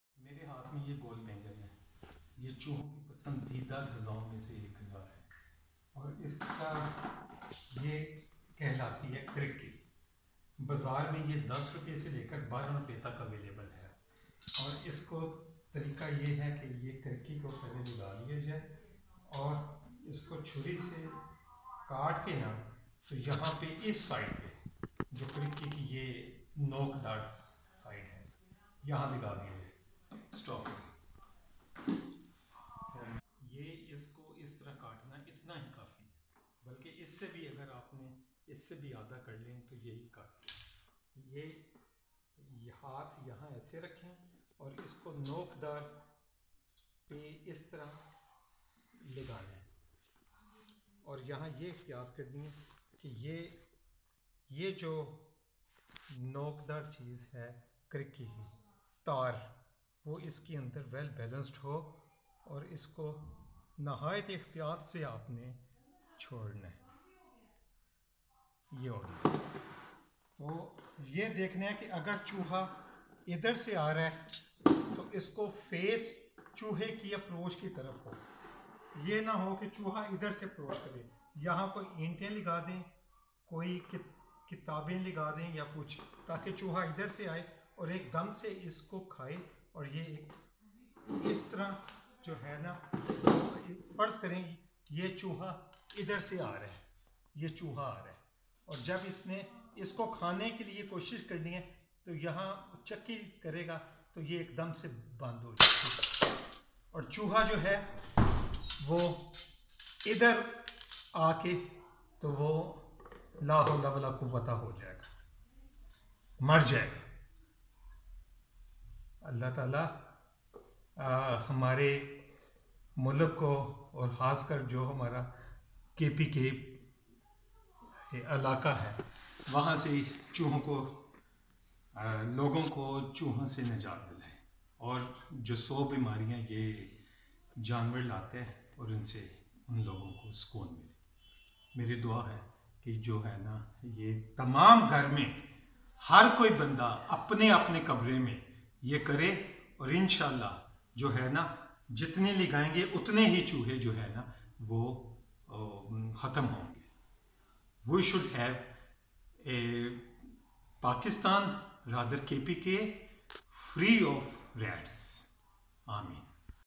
How to set traps for rats
Kpk province in Pakistan is facing rat problems. This video is to help those who want to get rid of these rats from their home.
May Allah help Pakistan to overcome this problem.